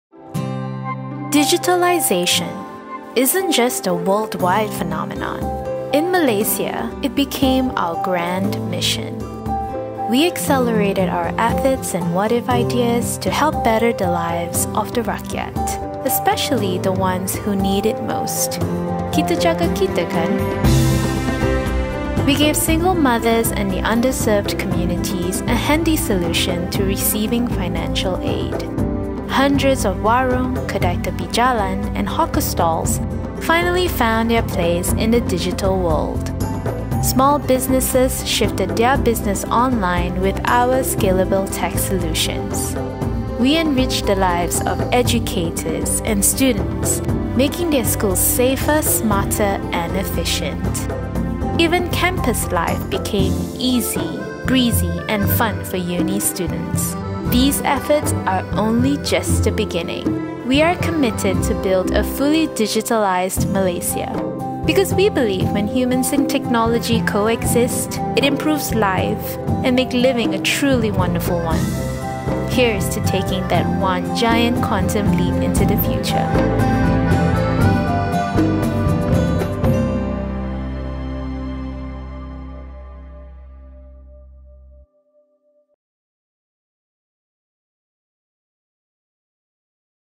Demi Malaysia
When humans and technology coexist, we can achieve amazing things. That’s why Green Packet is committed to building a fully digitalised Malaysia where underserved communities are enriched by life-improving innovations. Here’s to a better future for all - Demi Malaysia! Learn more about Green Packet’s digitalisation initiatives over the decades